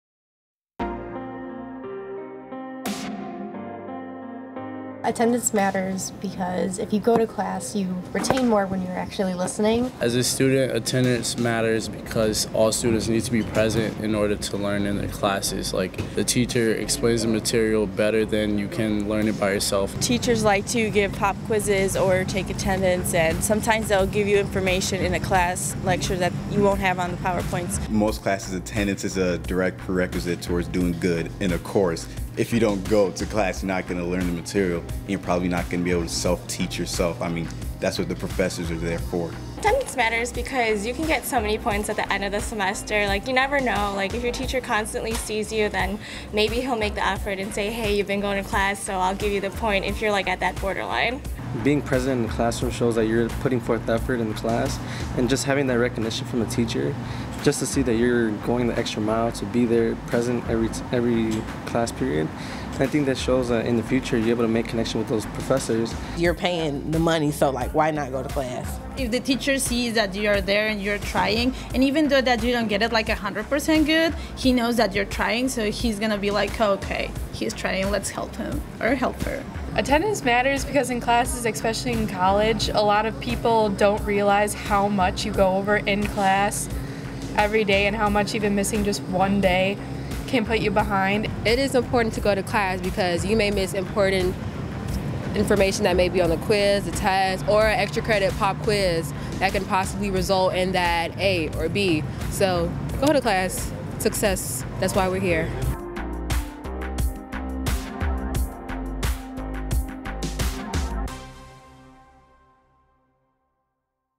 Why does attendance matter to NIU students?
NIU students explain the perks of attending class, from extra credit opportunities to getting what you paid for...these Huskies know their stuff.